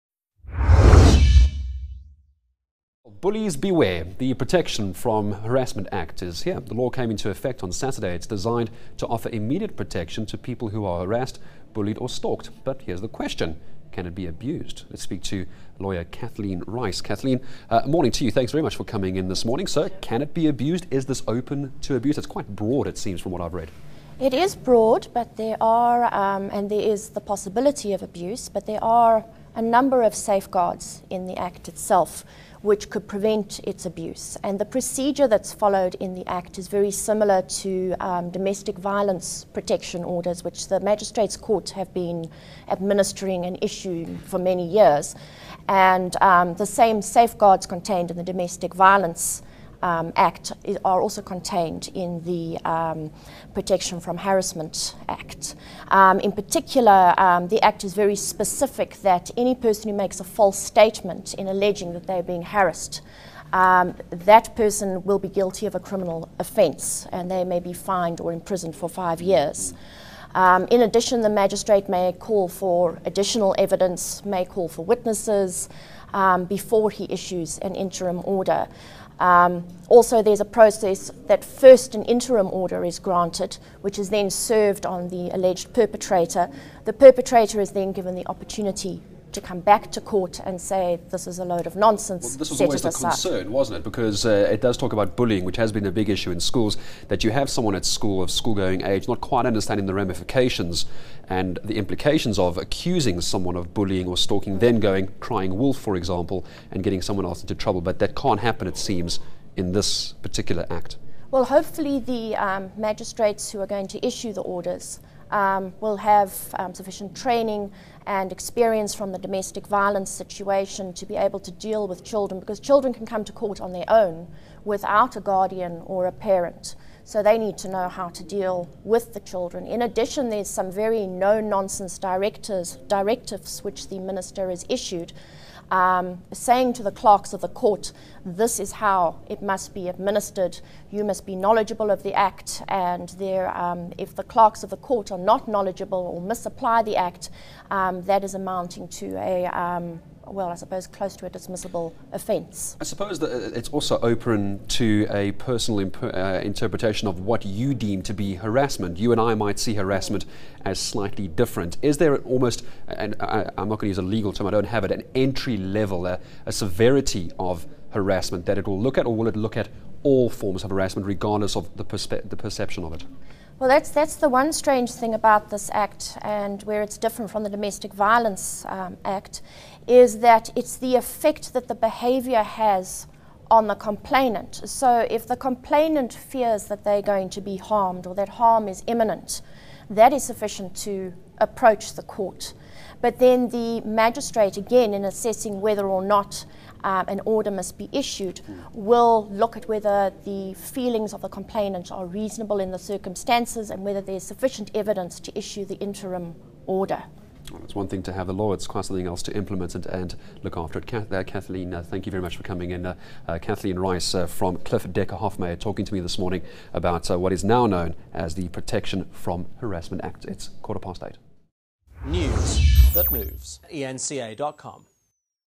Protection from Harassment act safe from abuse
Johannesburg, April 30 - Legal advisor Kathleen Rice says there are sufficient safeguards in the new Protection from Harassment Act to ensure that people do not abuse this new legislation.